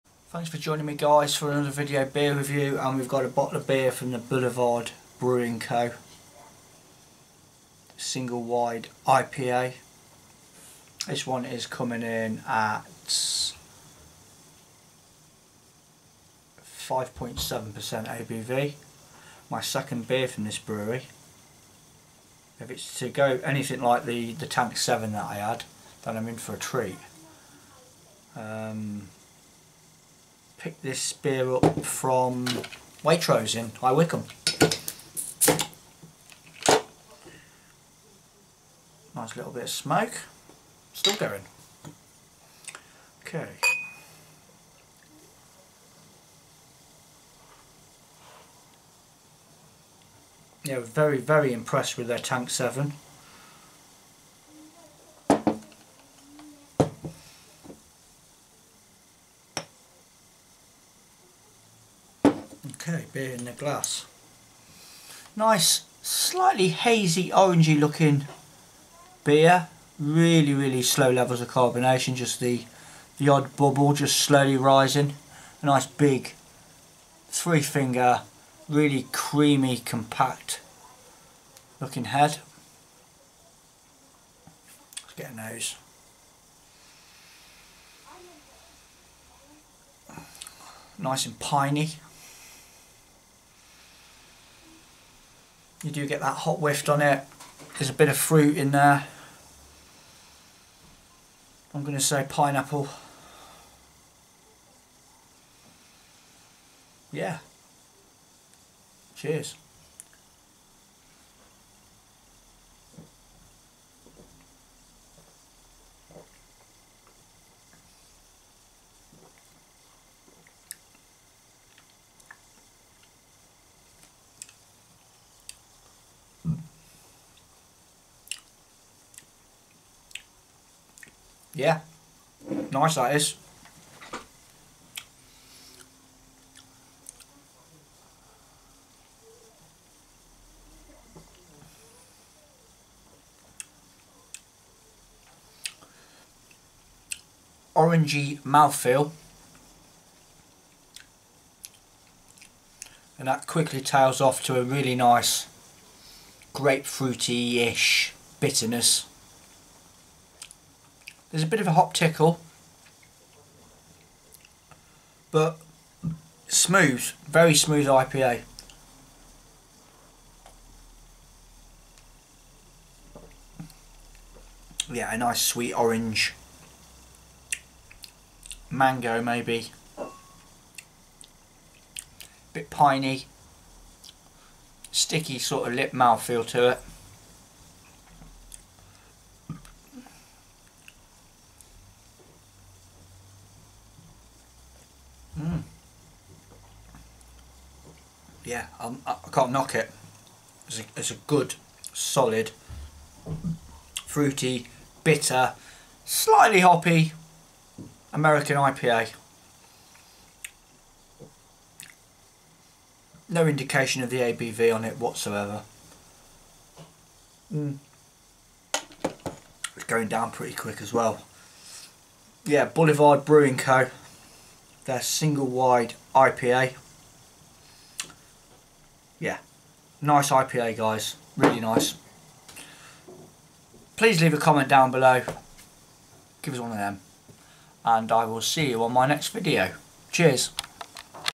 Boulevard Brewing Company (Duvel-Moortgat) | Single Wide IPA
COMMERCIAL DESCRIPTION
Boulevard Single-Wide I.P.A. is our take on a style that originated in 18th century Great Britain. This American version—inspired by our Smokestack Series Double-Wide I.P.A.—boasts a heady combination of six varieties of hops, some of which were employed for dry-hopping.